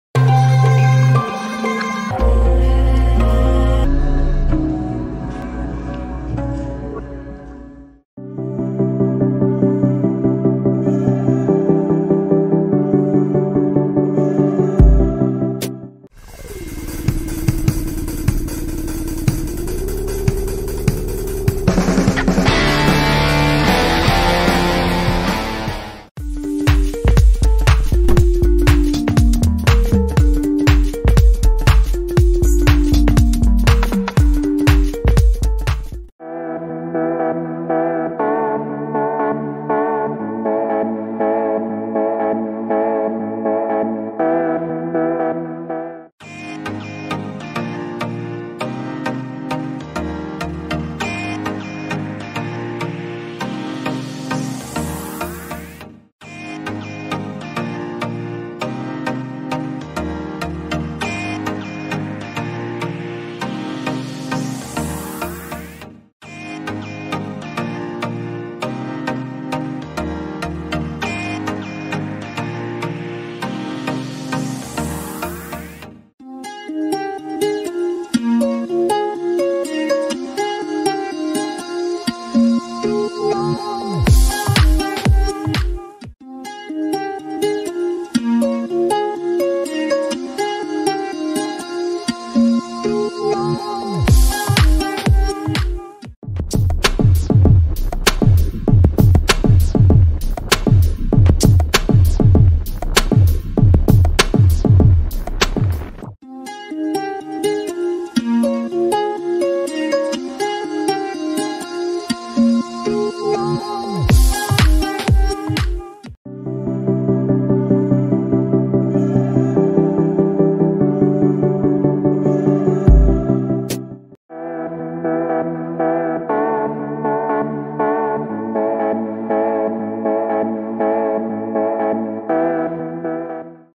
Wendy Schaal Top 10 Movies | Best 10 Movie of Wendy Schaal
Top 10 Movies of Wendy Schaal

Find the list of movies by Wendy Schaal you can find more information, movie trailers, posters, reviews, youtube links in below link.
Where the Boys Are '84

Pl. Comment down your favorite movie of Wendy Schaal